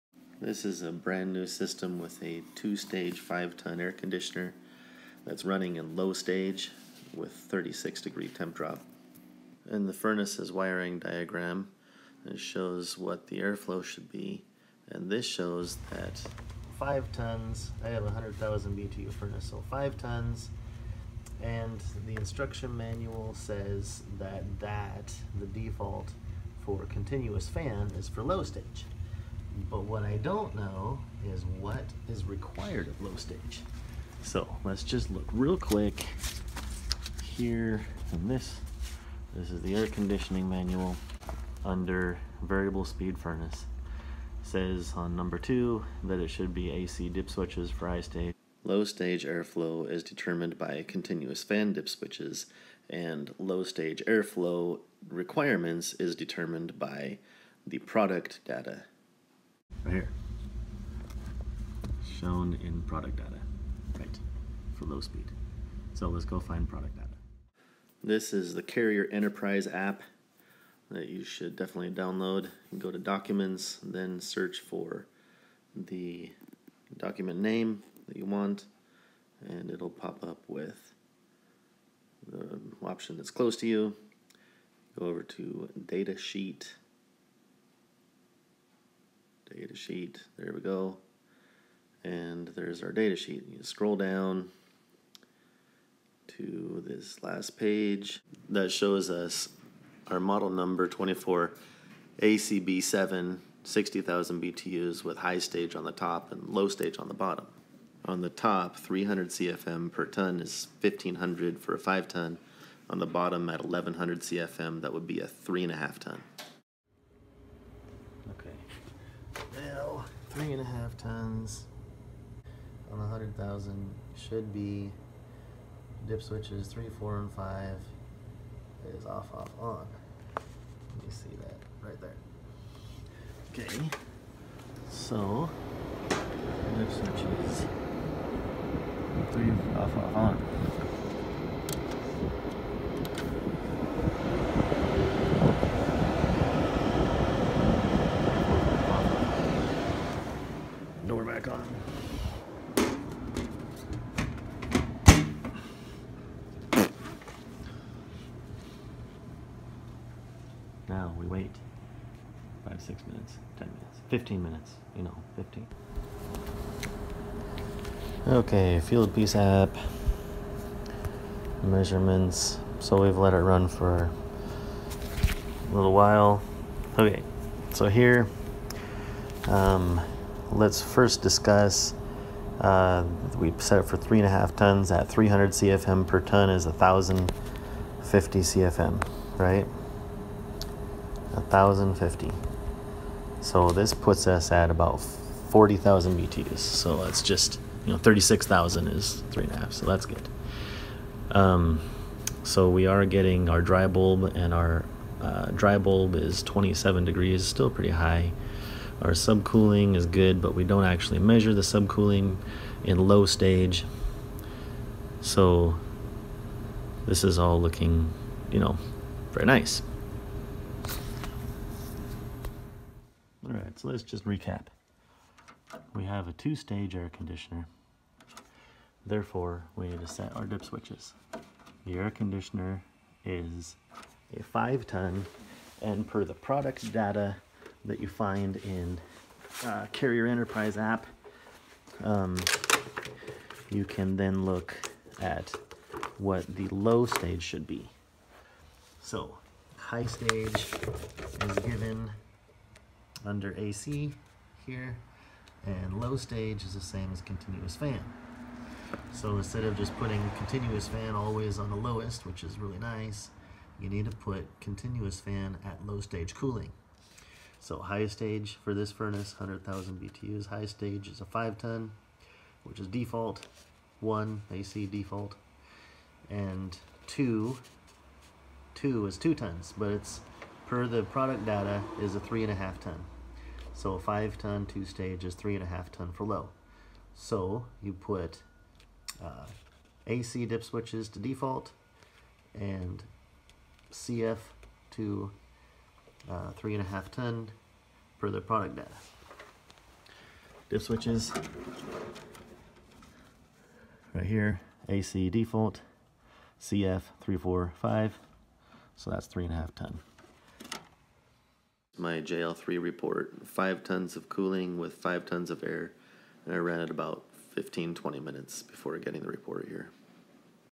2 stage AC and setting low stage airflow
The following is information that doesn’t just seem to be given to us, we have to hunt it down. So here I show where to find the info and how to set it up. The furnace is non communicating with a 2 stage heat/cool thermostat.
   I used Carrier Enterprise app, a free app that has all sorts of useful carrier stuff including service manuals, updated parts lists on older equipment, and here, the data sheet showing ideal airflow.
   I also used fieldpiece app with my fieldpiece JL3 refrigerant tools because they are super convenient.. real-time measurements from almost anywhere in the home, almost 0 refrigerant loss because of no hoses, and a printable report for the contractor’s and client’s records.
Please comment, tell me what I missed or did right/wrong.